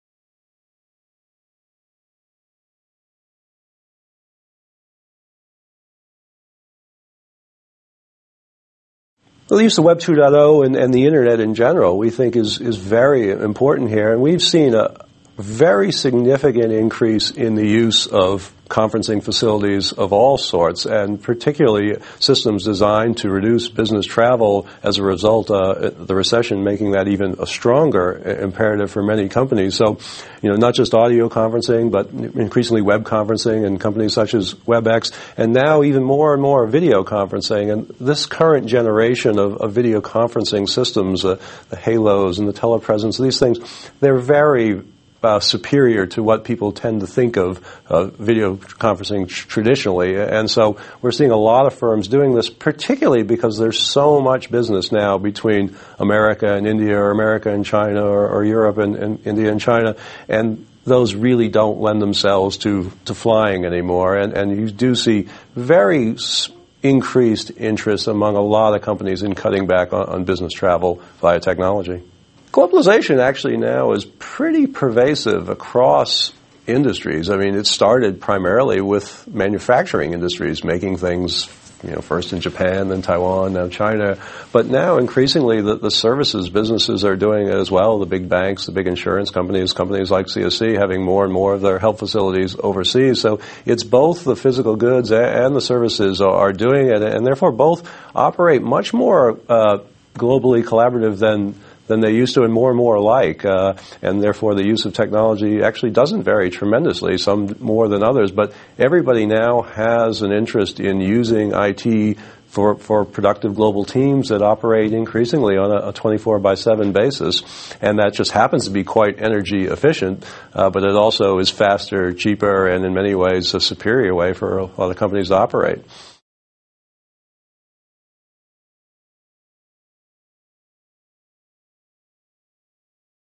Green IT: Leveraging Technology and Globalization
LEF Global Research Director, David Moschella discusses the report Green IT: Moving Beyond the 2% Solution(March, 2008). To fully understand the implications of Green IT, we had to go beyond the traditional focus on enterprise IT usage, and consider the energy-intensive processes used in IT hardware manufacturing as well as the energy-saving value of IT to the wider economy. We found that much of the current Green IT emphasis is misplaced. It focuses almost exclusively on data centres and PC power management, which together account for only about 2 percent of overall energy usage. While more attention must be paid to the considerable energy used in IT manufacturing, it is the huge potential energy savings that IT can bring to the wider economy that will create the most important business opportunities.